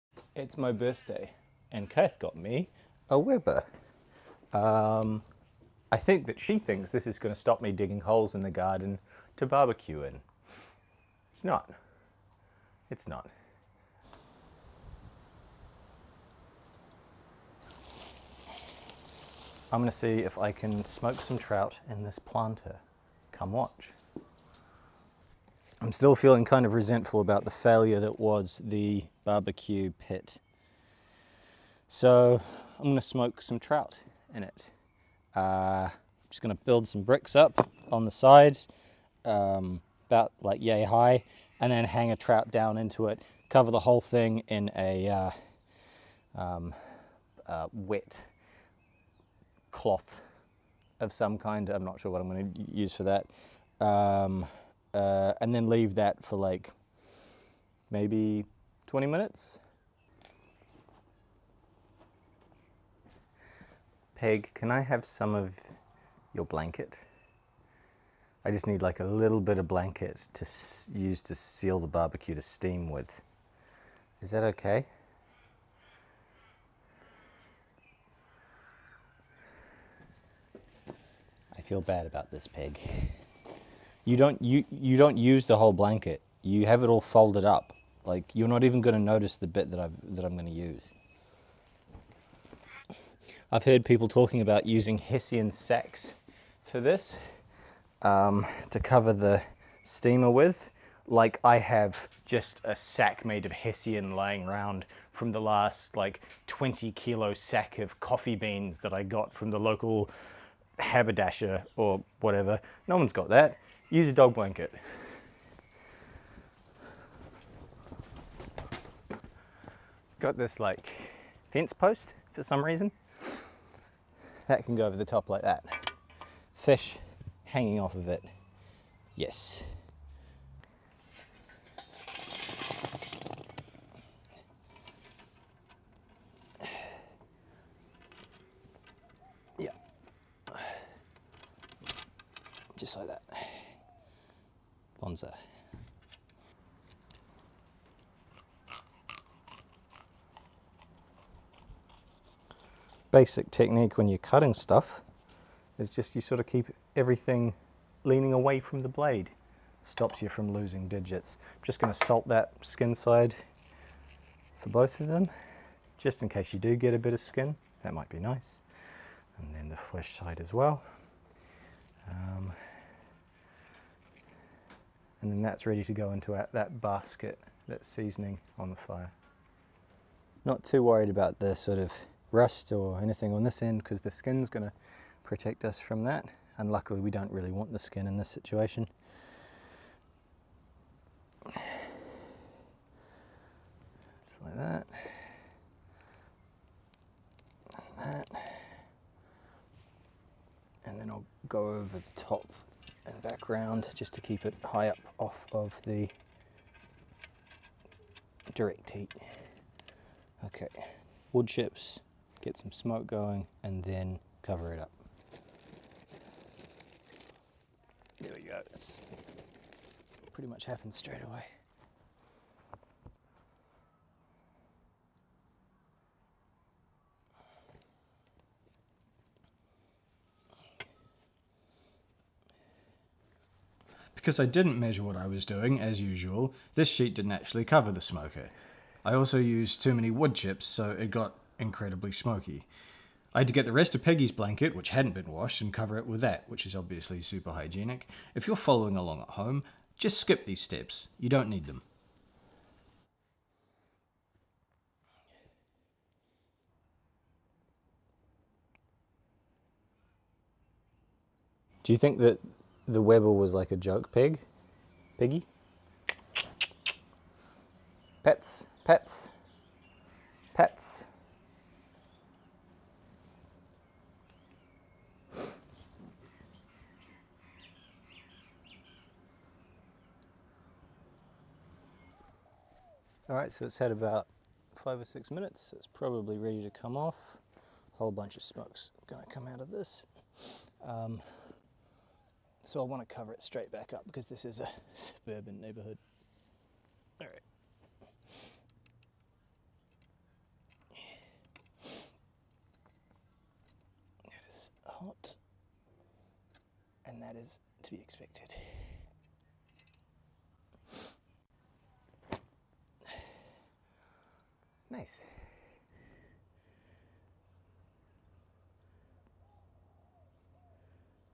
DIY Pit Smoker BBQ
My underground BBQ pit was pretty terrible, so I decided to turn it into a DIY pit smoker BBQ and do some trout. Trout are a a dry, unappealing  river fish and this is a great way to breath some life into the dull buggers.